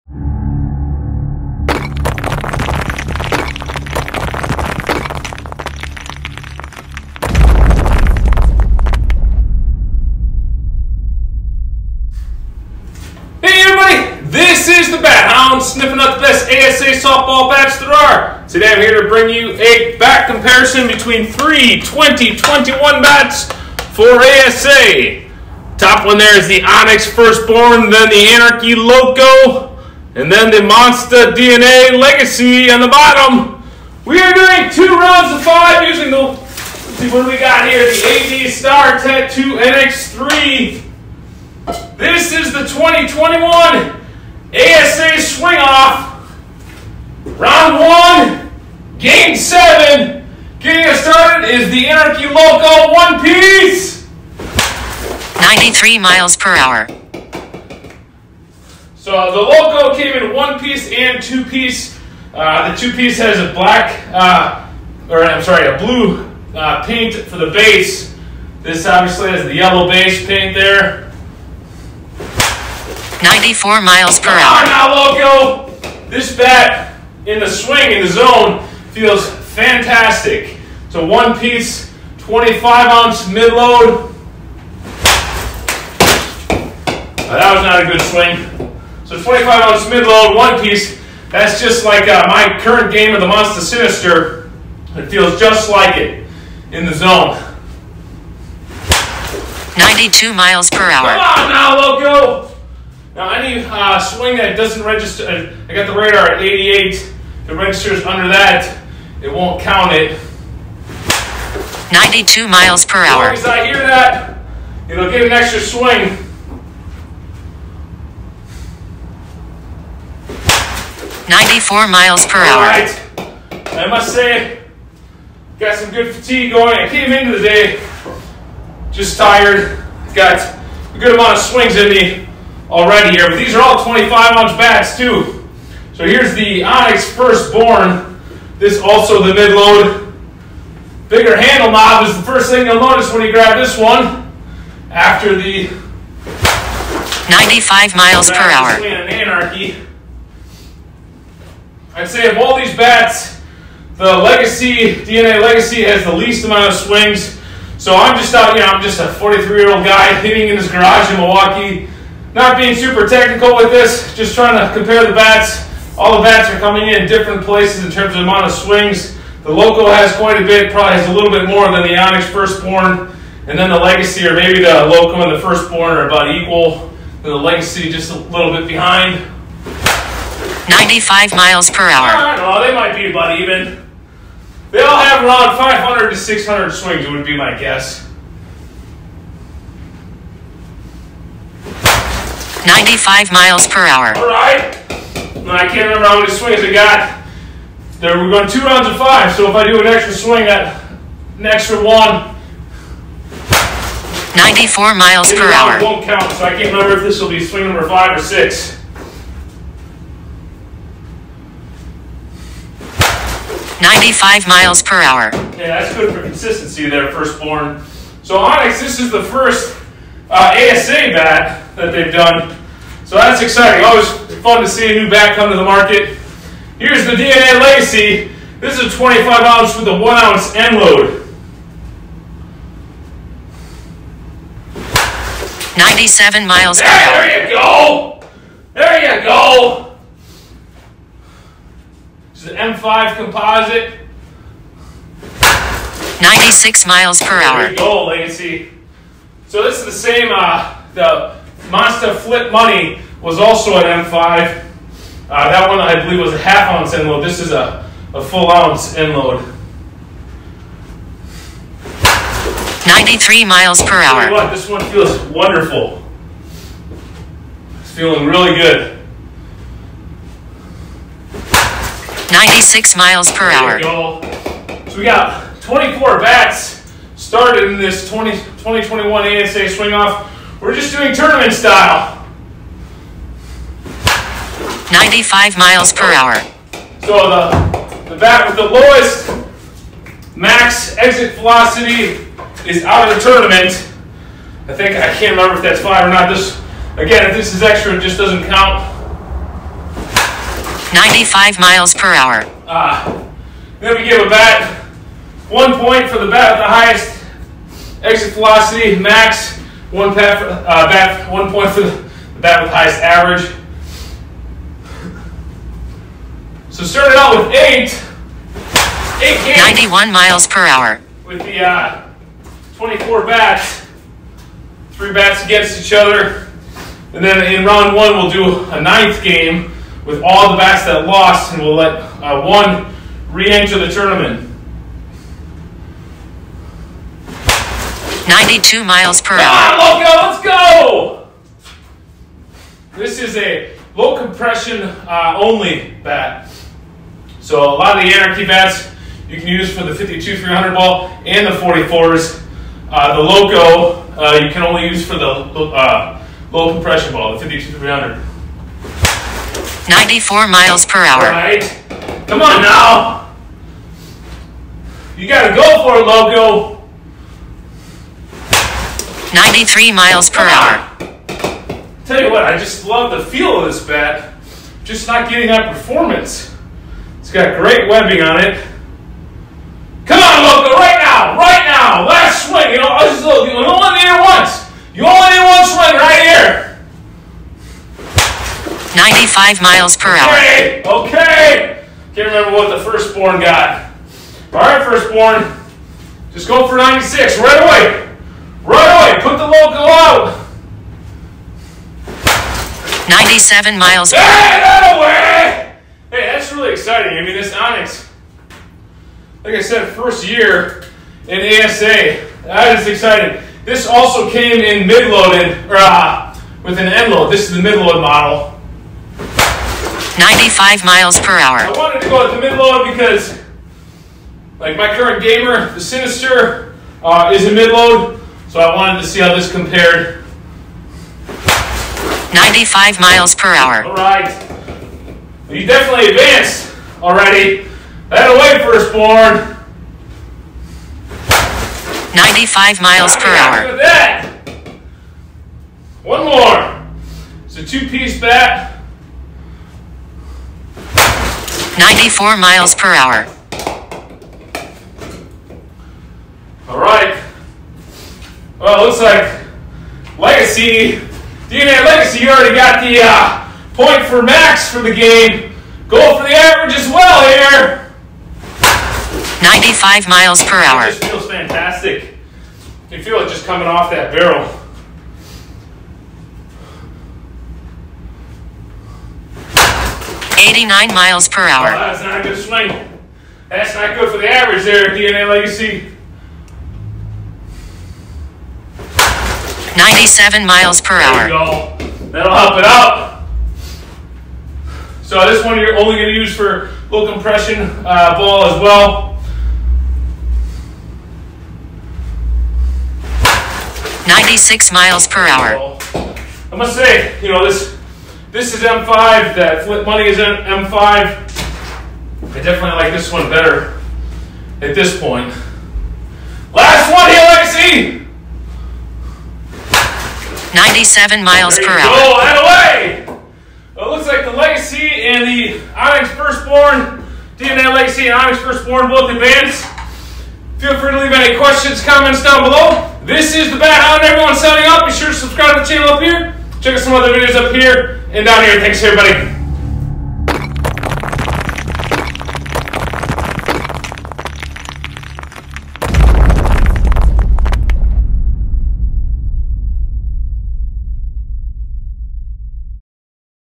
COMPARISON--Monsta DNA Legacy, Onyx Firstborn, Anarchy Loco 1pc (Rd 1, Game 7, 2021 ASA SWING OFF)
Here is a comparison of the 2021 Monsta DNA Legacy, Onyx Firstborn, Anarchy Loco 1pc (Round 1, Game 7, 2021 ASA SWING OFF).
POP TOSS GREEN PITCHING MACHINE